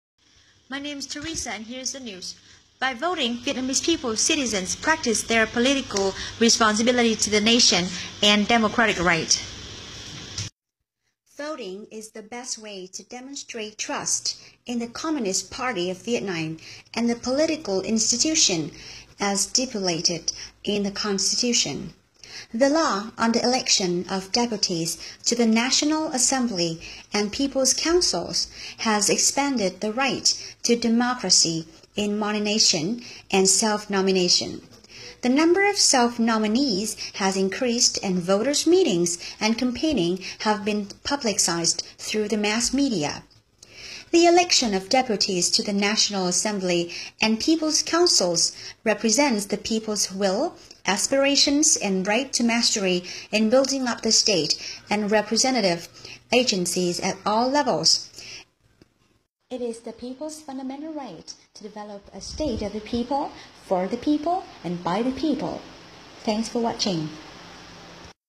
News 07 - Vietnam's election day - Trang Teresa
1. Political responsibility: trách nhiệm chính trị
2. Democratic right: quyền dân chủ
3. Communist Party: Đảng Cộng Sản
4. Political Institution: cơ quan chính trị
5. stipulate: quy định
6. The Constitution: Hiến pháp
7. The law on the Election: luật bầu cử
8. Deputy of National Assembly: Đại biểu quốc hội
9. People's Council: hội đồng nhân dân
10. nomination: đề cử
11. self-nomination: tự ứng cử
12. self-nominee: người tự ứng cử
13. mass media: thông tin đại chúng
14. will: ý muốn
15. aspiration: khao khát
16. fundamental right: quyền cơ bản
17. the state: chính phủ
18. of the people, for the people, by the people: của dân, do dân và vì dân

Script:
National Election day
Hi, my name is Teresa and here is the news.
By voting Vietnamese people citizens exercised their democratic right and fulfilled their political responsibility to the nation. Voting is the best way to demonstrate trust in the Communist Party of Vietnam and the political institution as stipulated in the Constitution.
The Law on the Election of Deputies to the National Assembly and People’s Councils has expanded the right to democracy in nomination and self-nomination. The number of self-nominees has increased and voters’ meetings and campaigning have been publicized through the mass media.
The election of deputies to the National Assembly and People’s Councils represents the people’s will, aspirations, and right to mastery in building up the state and representative agencies at all levels. It is the people’s fundamental right to develop a state of the people, for the people, and by the people.